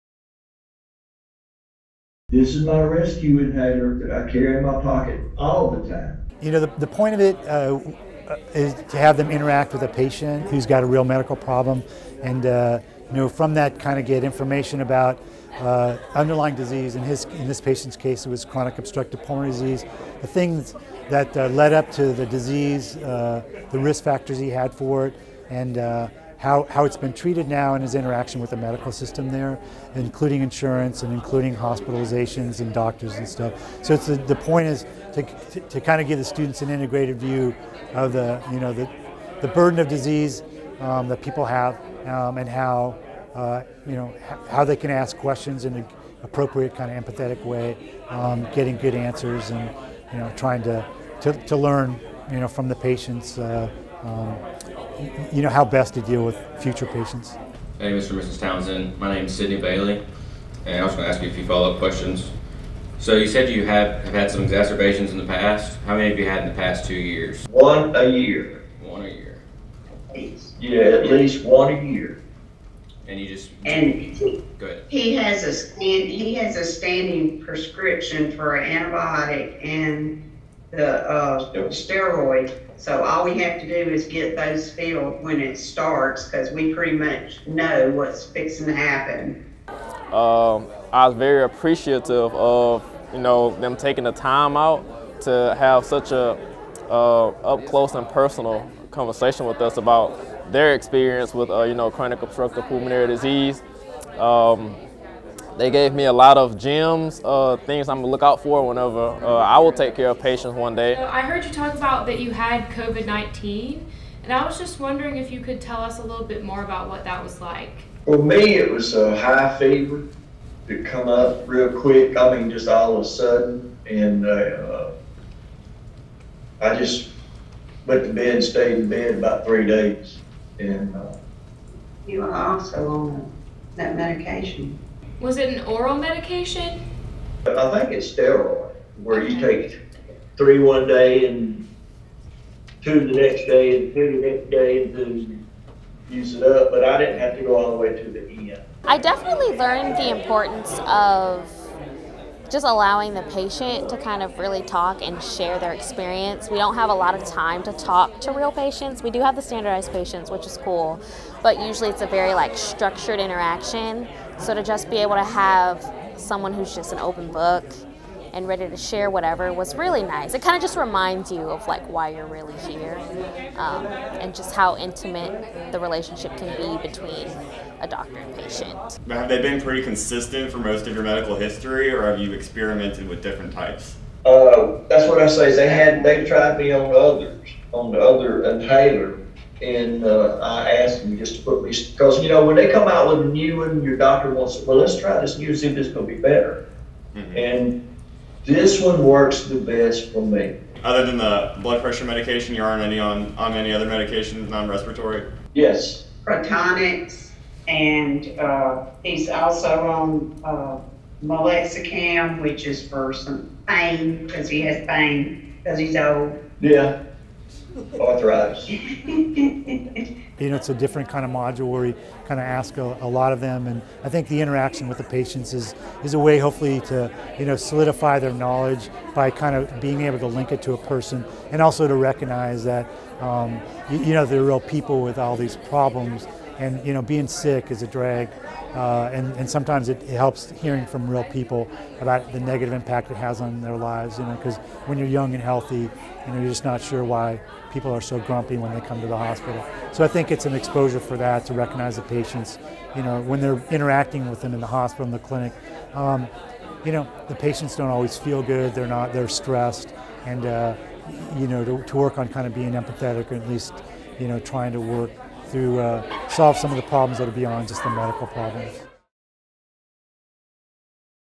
Whiddon College of Medicine Respiratory Module
This video features first-year medical student interactions with a patient during the Respiratory Module at the Frederick P. Whiddon College of Medicine at the University of South Alabama.